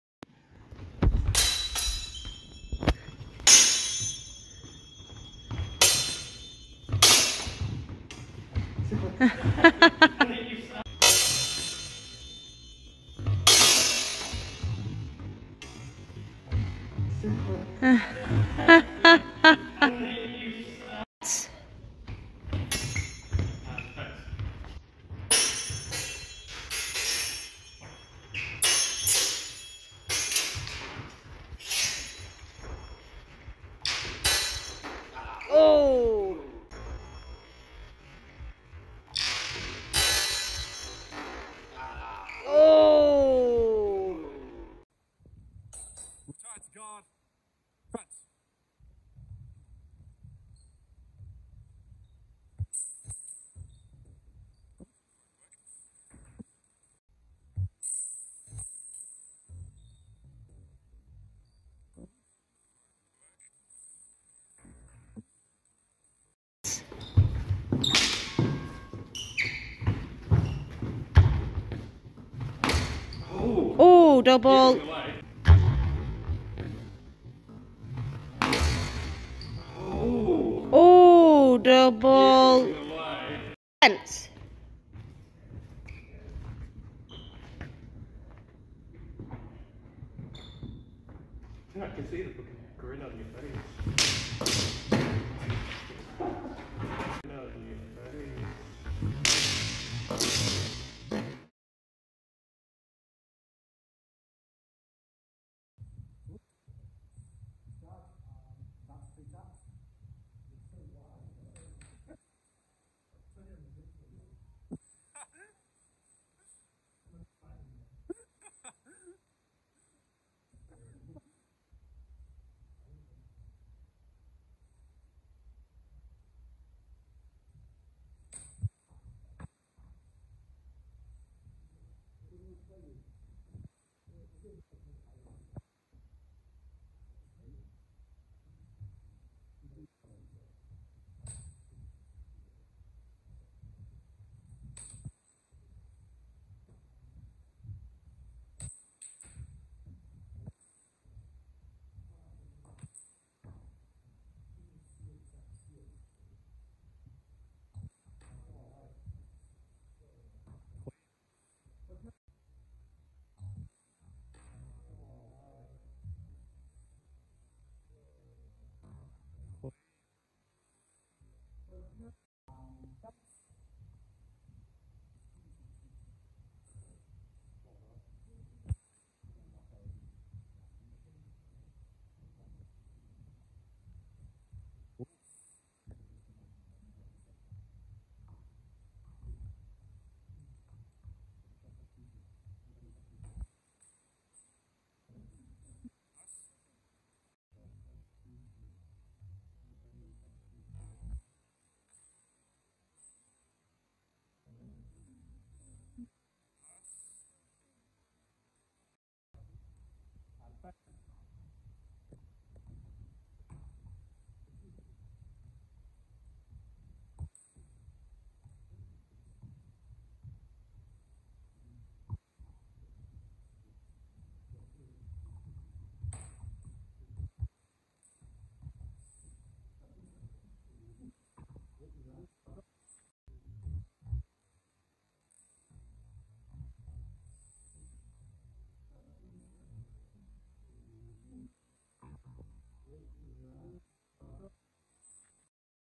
Steel Longsword Sparring
Some steel longsword sparring, mainly inspired by the works of Joachim Meyer and Fiore